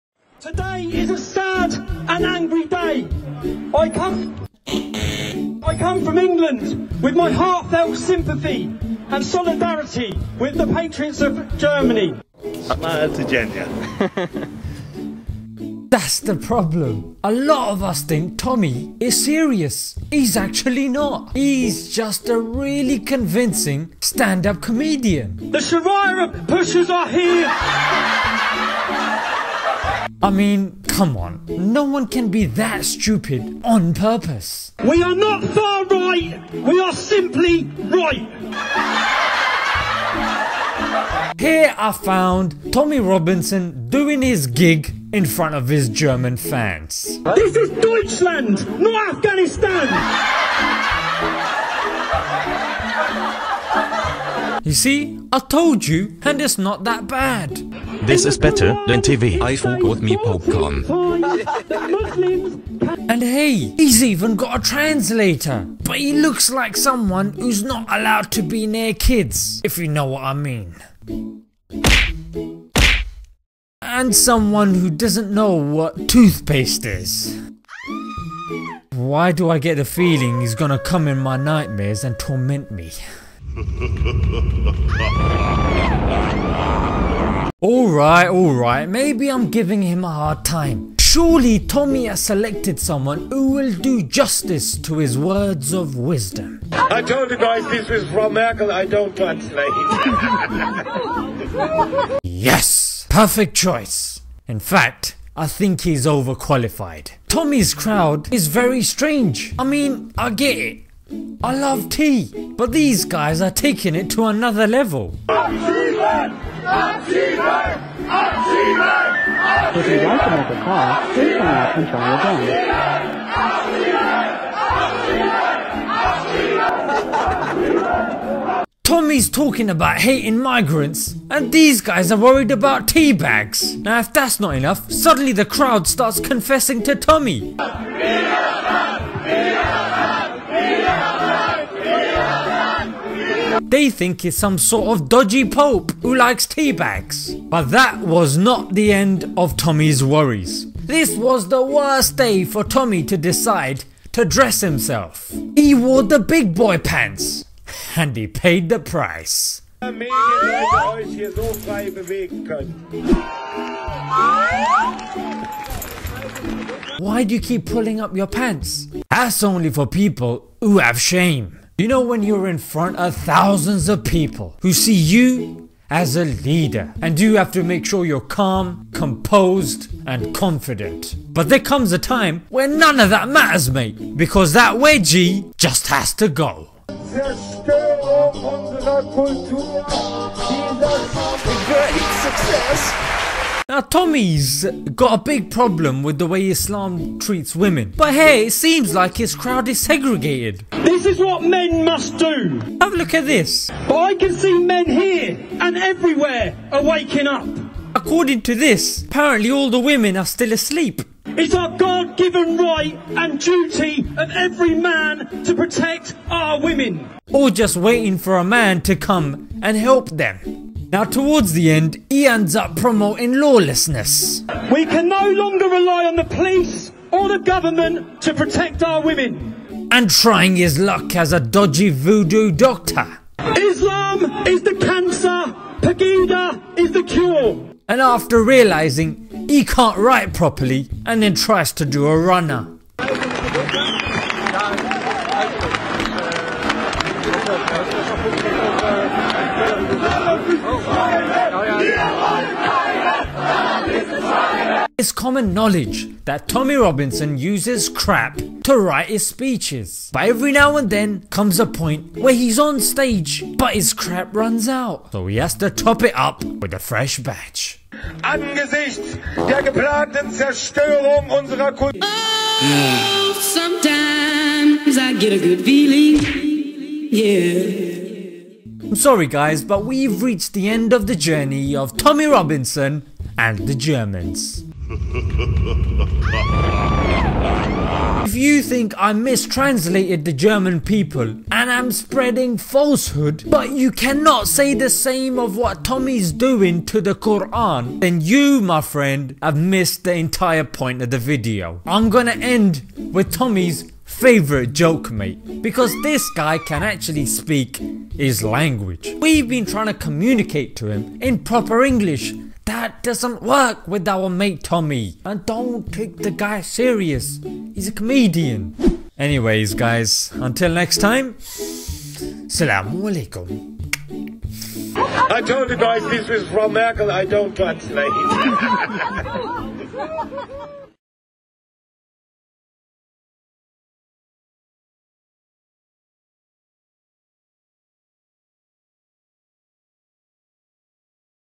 TOMMY ROBINSON DOES STAND UP COMEDY
Tommy Robinson does his best stand up routine to date.